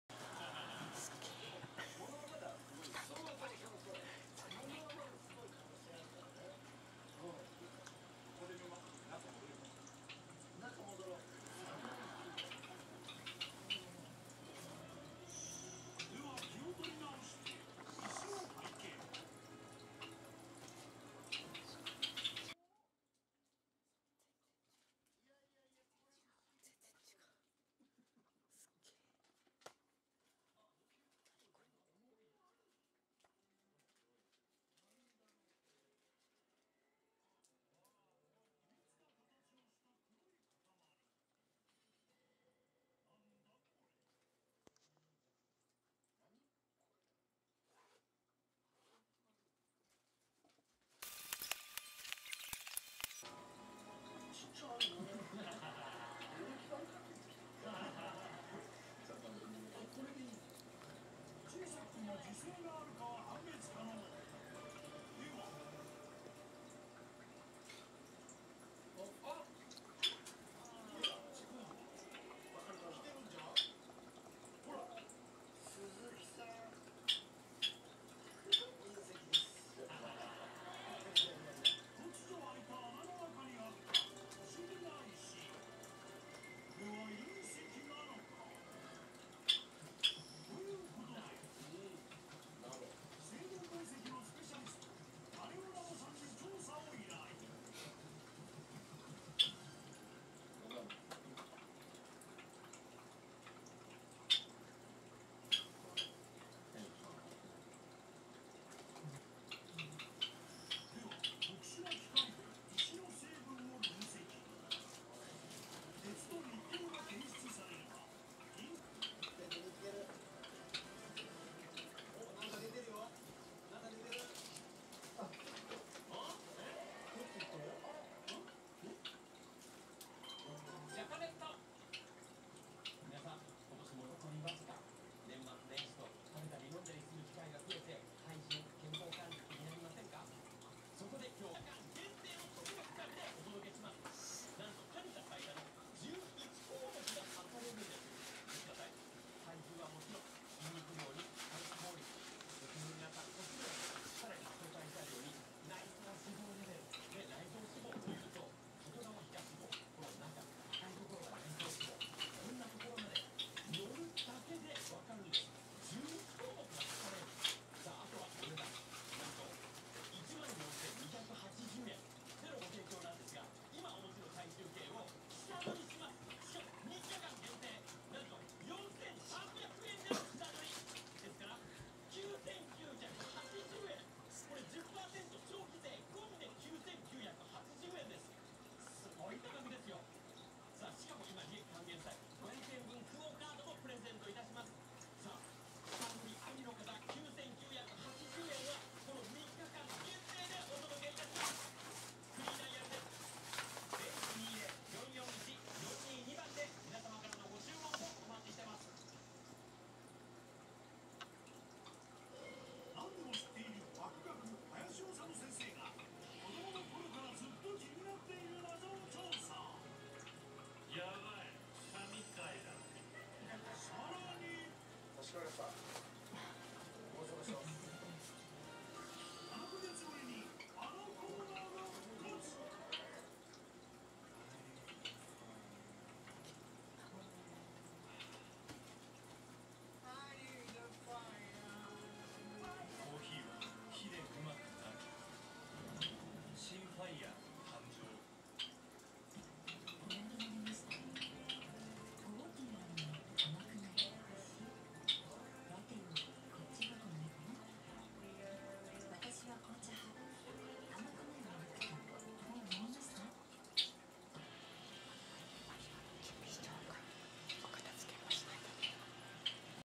まいるどライフ 全員集合 久々のお気に入りスポット「カーテン」
皆のお気に入りスポとカーテンの上でくつろぐ文鳥さんたち

文鳥雛 成長の記録
我が家に来て47日目の雛たちと先輩たちの様子です